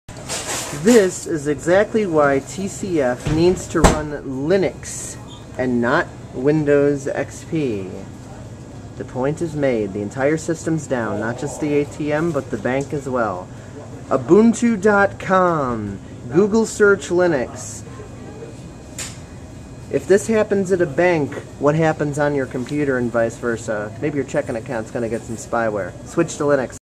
Xpresit.net - Crashing TCF Bank Locations Since 1995
The other week, my TCF National Bank Location that I use was DOWN due to some bizarre Windows XP Dysfunction. I know this, because I asked them what the problem was.

I told the lady that they need to switch to Linux. None of the issues that Windows XP experiences. Open Source so they can save millions by being able to have as many installs as they want -- and do whatever they want with it -- instead of being subject to Microshaft's Licensing Fees, Developer Kit Subscriptions, etc.

Microsoft: What TCF Bank Location Do You Want Crashed Today?